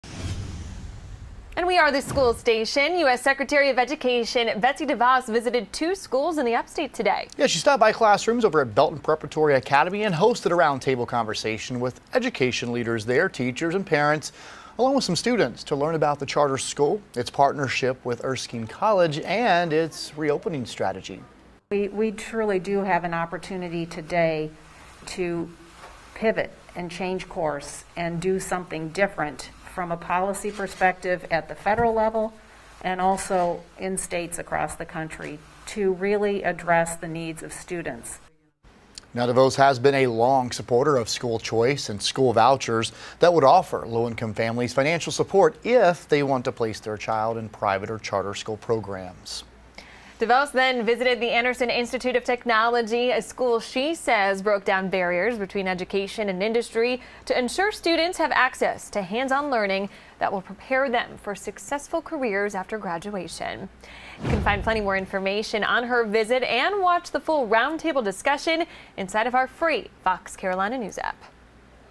U.S. Education Secretary Devos visits two area schools
Betsy DeVos visited Belton Preparatory Academy and the Anderson Institute of Technology on Tuesday